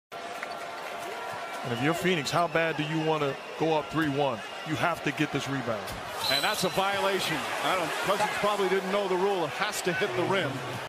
DeMarcus Cousins Chokes Game 4 Against Suns 🤣
Demarcus Cousins gets a free throw violation in the clutch